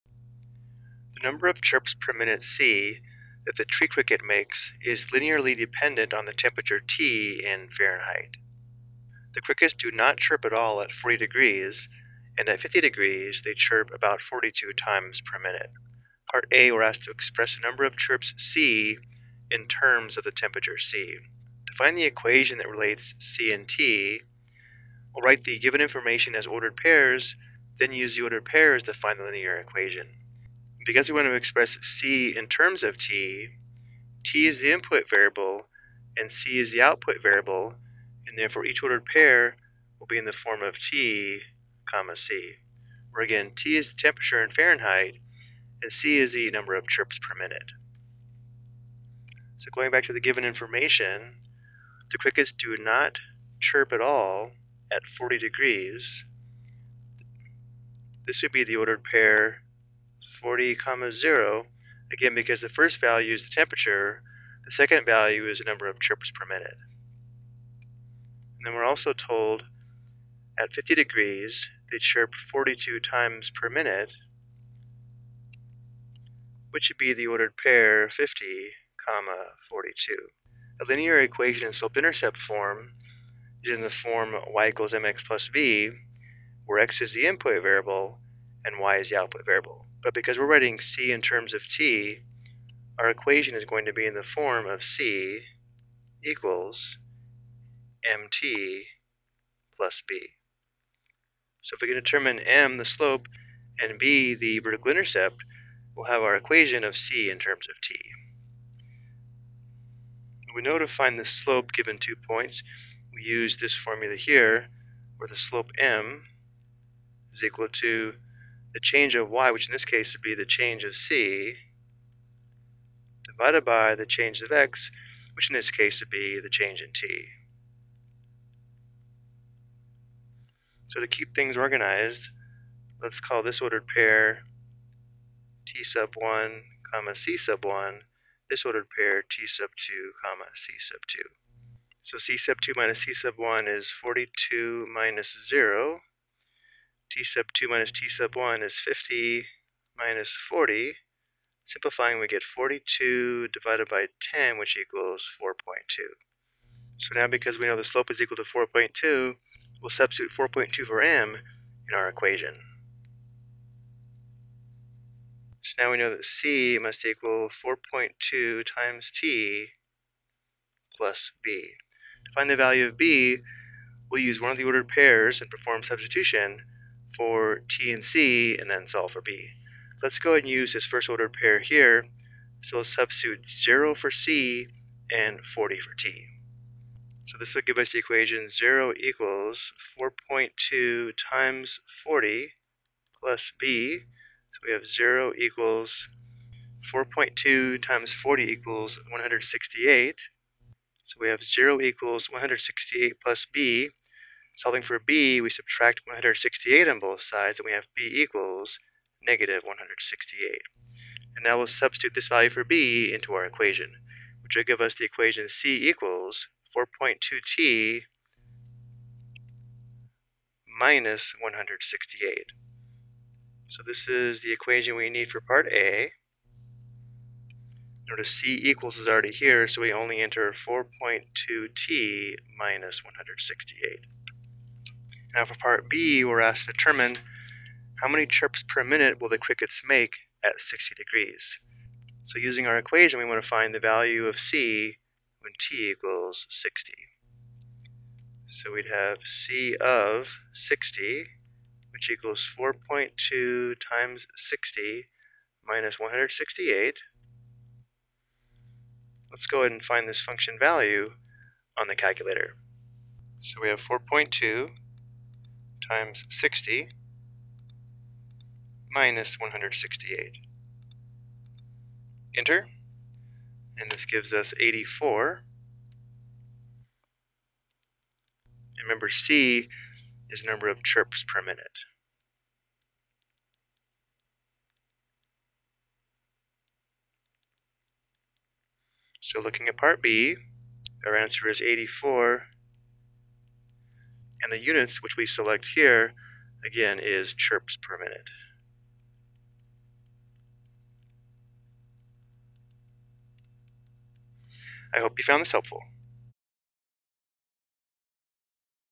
Ex: Write a Linear Equation that Models Cricket Chirps (Linear Equation Application)
This video explains how to write a linear equation that models a real life situation from given information.